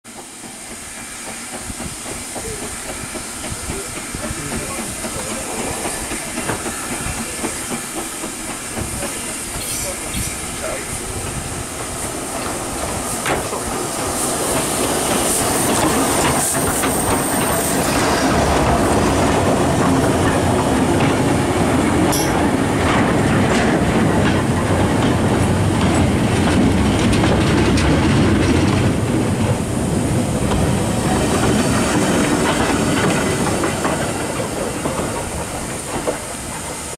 (Steam Train) B12 8572 Arrives Holt P2 (23/06/2018)
Published on Saturday 29th September 2018 (29/09/2018)